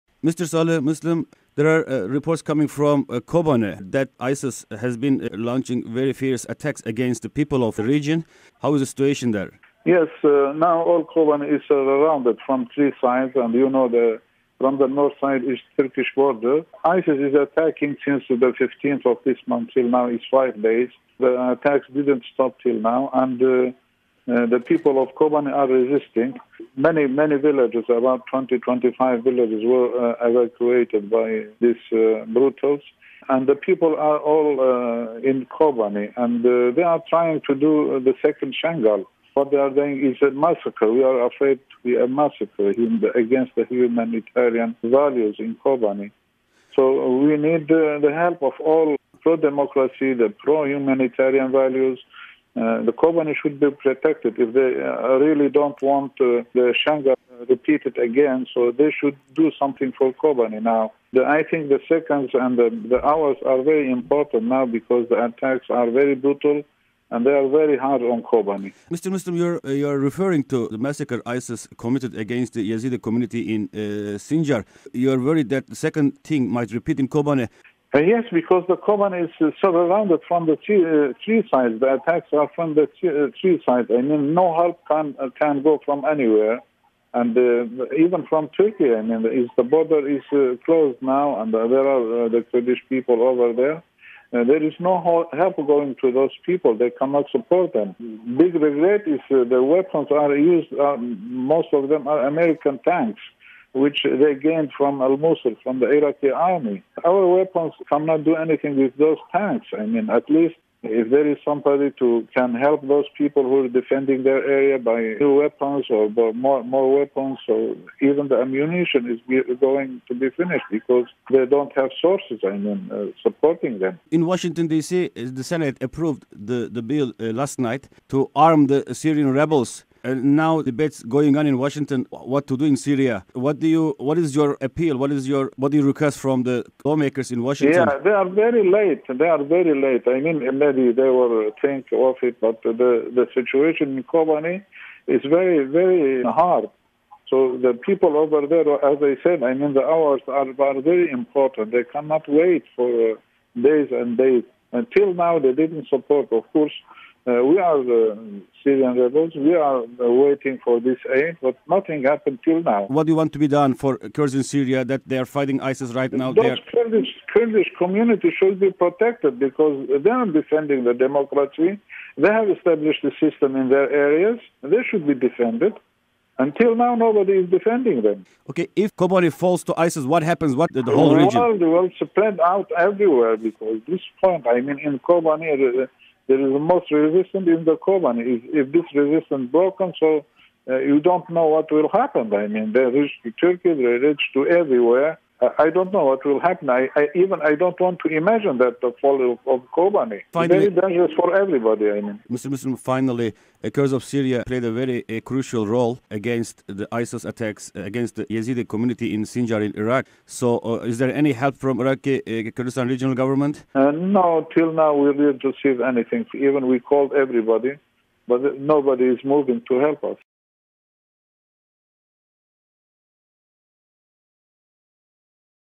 PYD Leader Salih Moslem on Kobane
PYD Leader Salih Moslem Appeal International Community to Protect  Kobane from ISIS Attacks